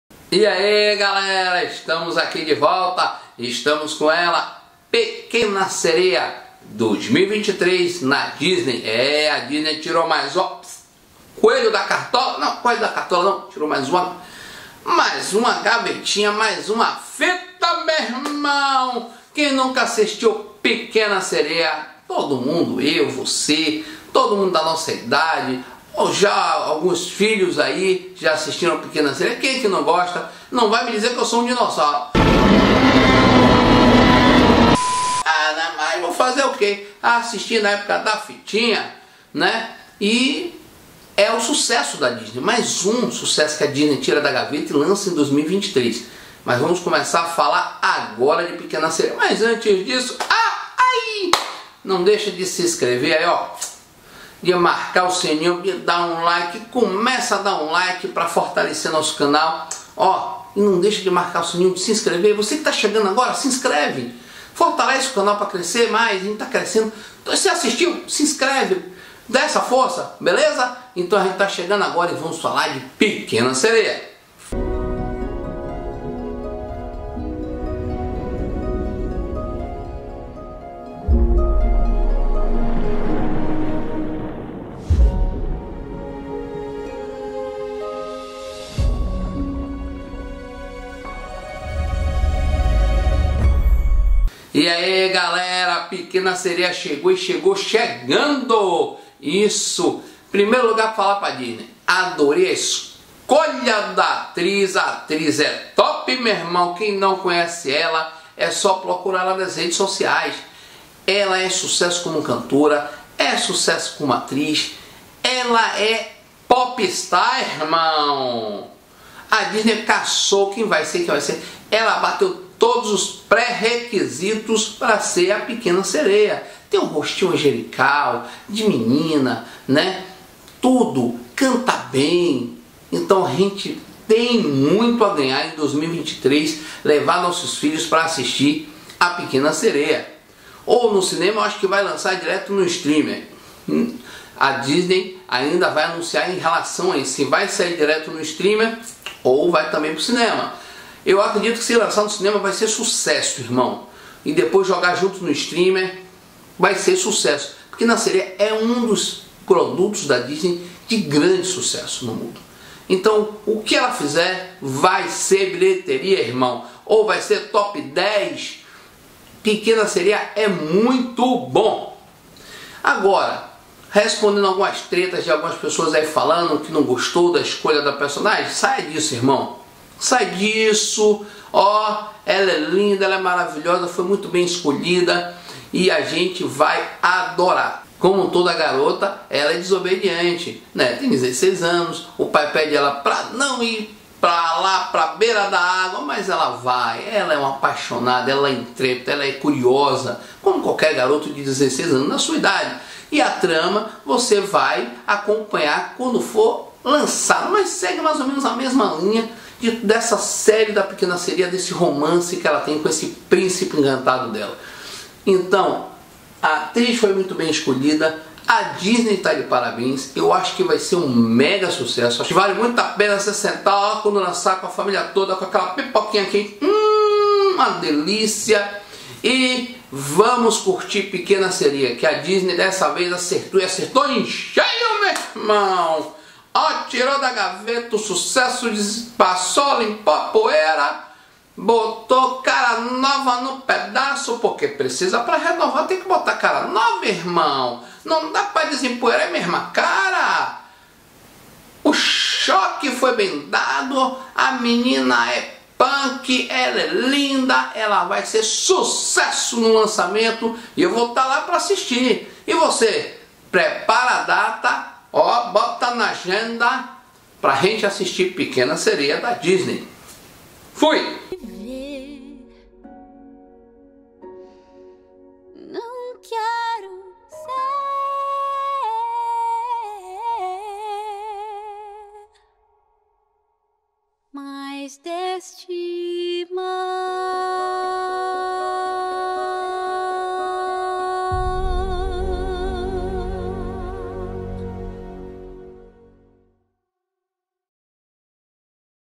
O FILME A PEQUENA SEREIA VAI FRACASSAR OU VAI SER O FILME DO ANO DA DISNEY
Uma jovem sereia faz um acordo com uma bruxa do mar, para trocar sua bela voz por pernas humanas para que ela possa descobrir o mundo acima da água e impressionar um príncipe.

Data de lançamento : 26 de maio de 2023 ( EUA )
Diretor : Rob Marshall
Adaptado de : A Pequena Sereia
Fotografia : Dion Beebe
Produtores : Lin-Manuel Miranda , Rob Marshall , Marc Platt
Música composta por : Lin-Manuel Miranda , Alan Menken